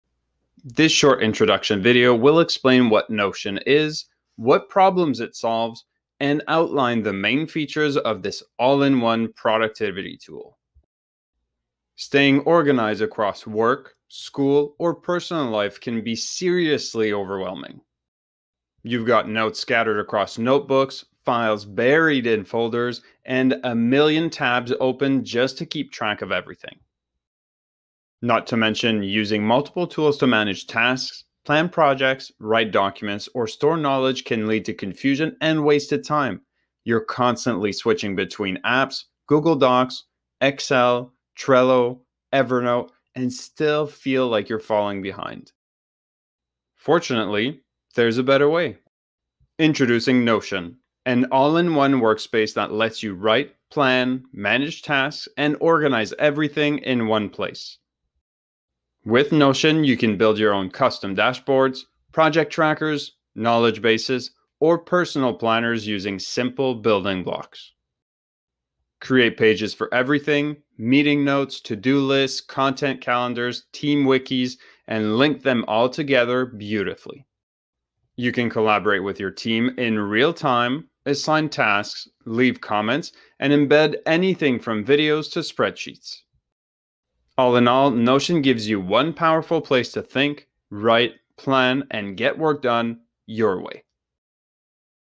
What is Notion? - Tool Explained [2025]
What is Notion? | A Simple 2-Minute Explanation

Feeling overwhelmed trying to manage your tasks, notes, and projects across too many apps? In this short video, you'll discover how Notion helps you stay organized, productive, and in control—all in one place.

🔍 In just 2 minutes, you'll learn:

The common problems people face with scattered tools

What Notion is and how it works

Why millions are switching to Notion for personal and team productivity

Whether you're a student, freelancer, or small business owner, Notion can simplify your life and boost your workflow.

✨ Watch now to see how Notion helps you think, plan, and get things done—your way.